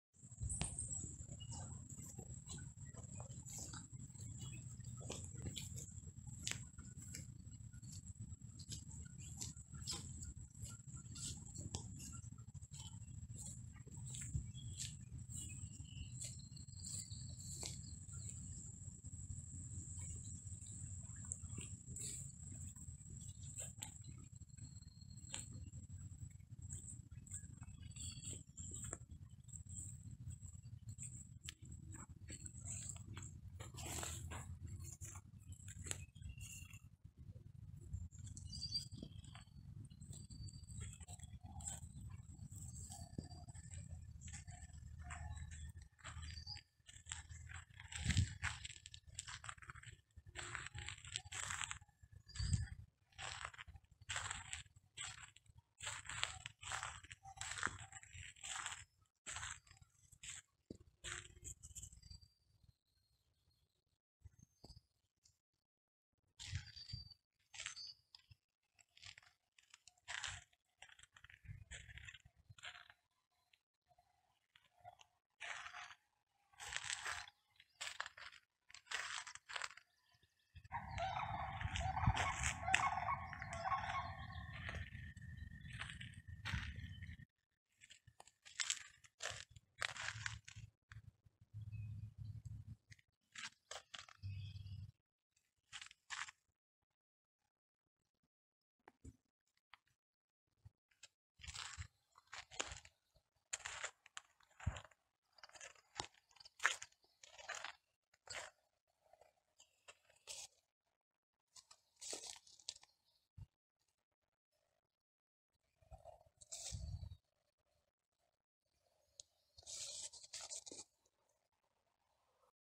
Interesting Tree on the Trail, This Stump Looks Just Like a Toilet
Found this interesting tree stump on the trail and, funny enough, it looks like a toilet.